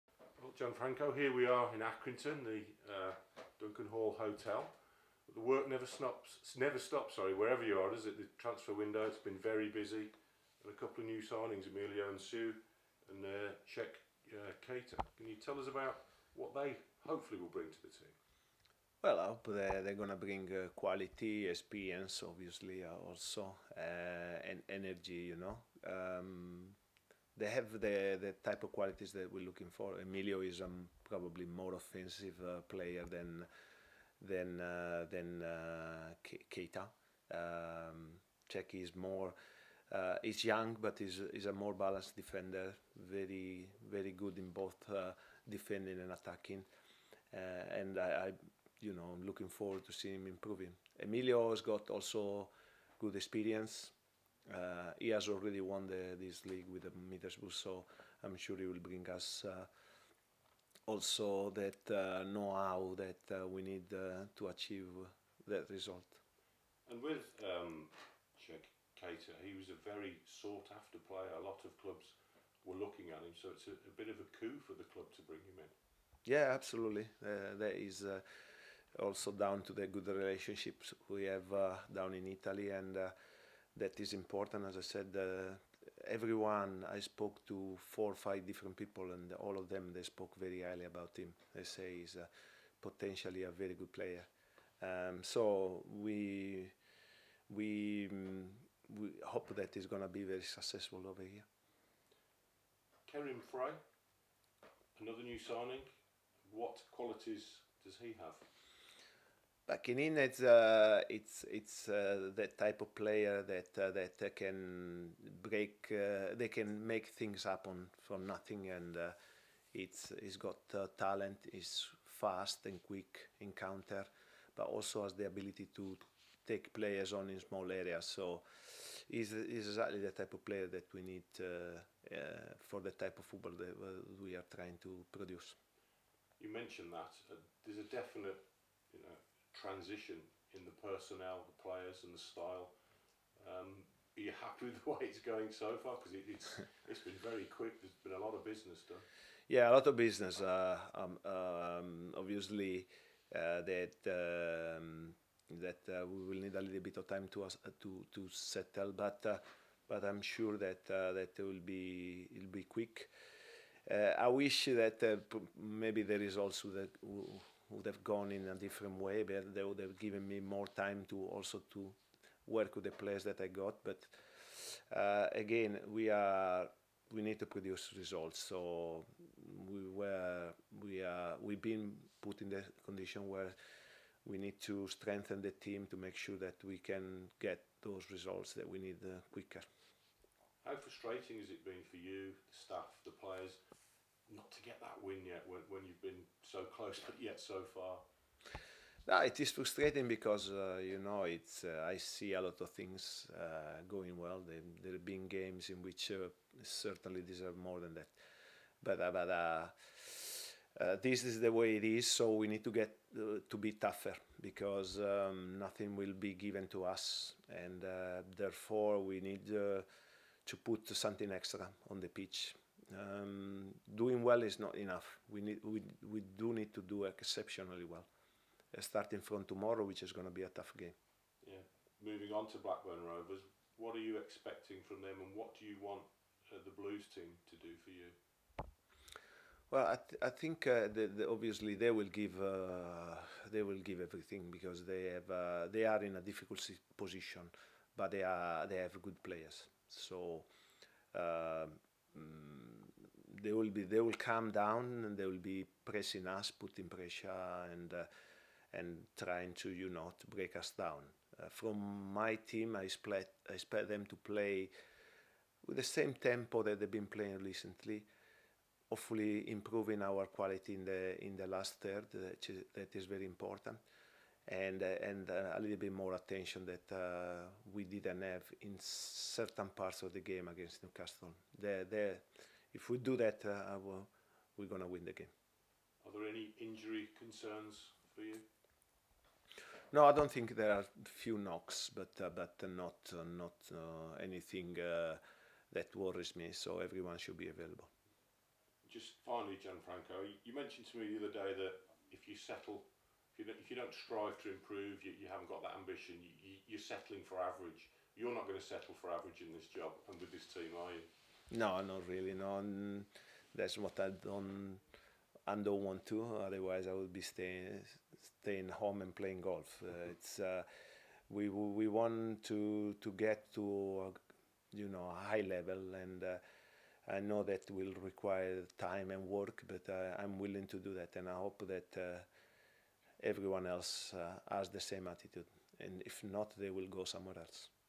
Gianfranco Zola previews Blackburn Rovers v Blues
Gianfranco Zola spoke to Colin Tattum ahead of Saturday's match at Blackburn Rovers.